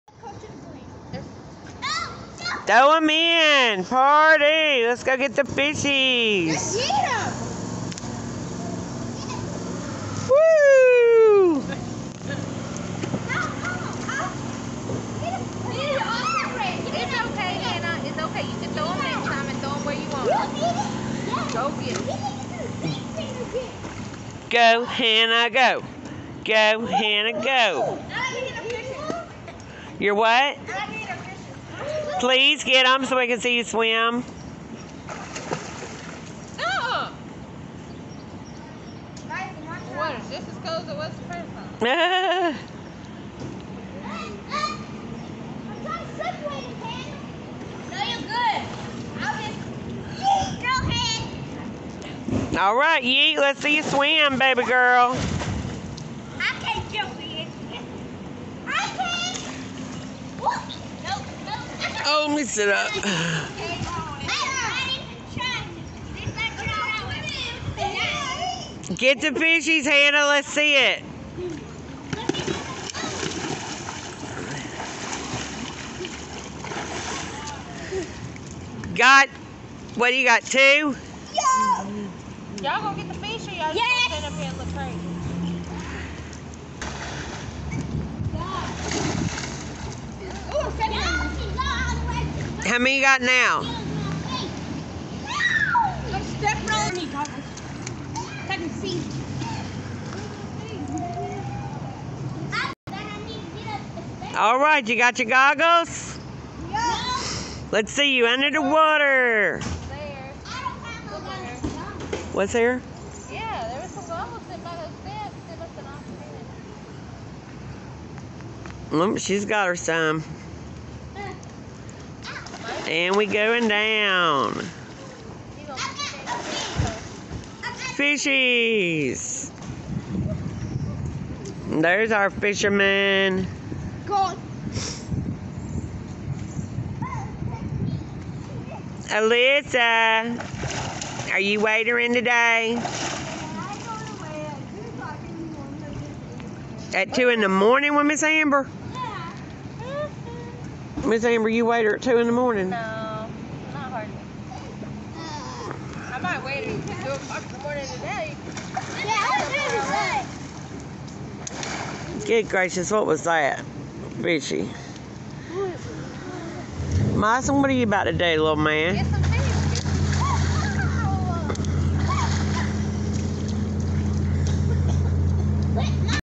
Kids playing at pool
Summer 2020. Took the kids to the hotel to swim and cool off. Hannah swims under water to get weighted fish.